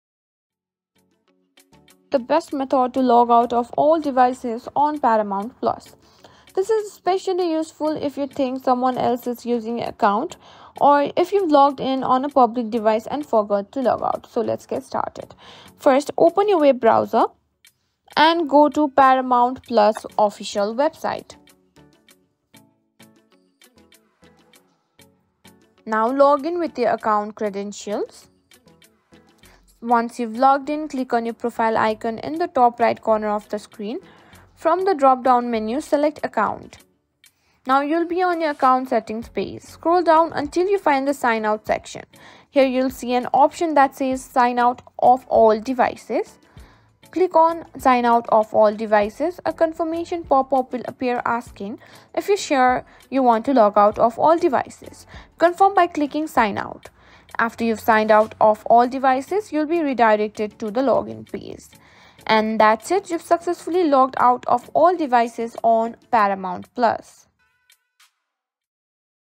How To Log Out Of All Devices On Paramount Plus (Best Method)
Quick Steps on How To Log Out Of All Devices On Paramount Plus. Learn How do I remove Paramount Plus from other devices with the steps on this tutorial video.

Struggling to log out of Paramount+ on forgotten devices? You're not alone! This video from @AppMastery  will show you the quickest way to sign out of ALL devices on your Paramount+ account, keeping your streaming secure.

Timestamps:

00:00:00 - Introduction
Learn how to log out of all devices on Paramount Plus.

00:00:05 - Open Web Browser
Open your web browser and go to the Paramount Plus official website.

00:00:10 - Log In
Log in with your account credentials.

00:00:15 - Access Account Settings
Click on your profile icon in the top right corner and select "Account" from the drop-down menu.

00:00:25 - Find Sign Out Section
Scroll down to the "Sign Out" section on the account settings page.

00:00:30 - Sign Out of All Devices
Click on "Sign Out of All Devices" and confirm the action.

00:00:40 - Confirmation
Confirm the prompt to sign out of all devices.

00:00:50 - Redirect to Login Page
You’ll be directed to the login page after signing out of all devices.

00:01:00 - Conclusion
You’ve successfully logged out of all devices on Paramount Plus.